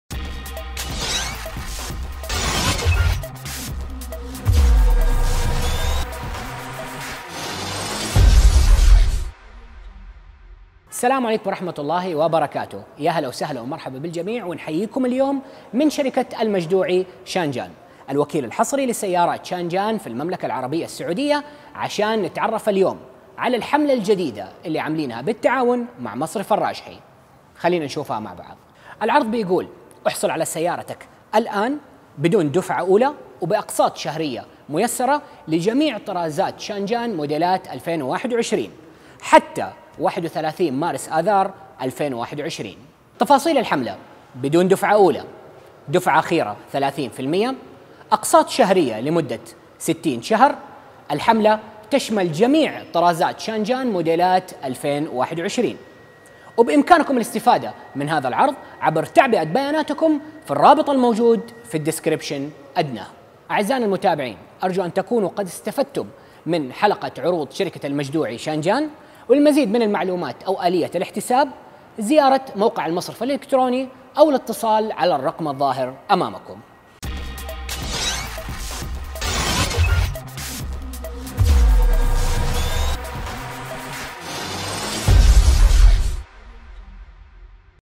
عروض شانجان بالتعاون مع مصرف الراجحي Changan Offers
شاهد هذا التقرير الذي نستعرض فيه الحملة الجديدة التي أطلقتها شركة المجدوعي للسيارات وكلاء سيارات شانجان في المملكة العربية السعودية بالتعاون مع مصرف الراجحي، ولمعرفة تفاصيل هذه الحملة شاهد هذا الفيديو.

للاستفادة من هذه العروض قم بتعبئة بياناتك عبر الضغط على هذا الرابط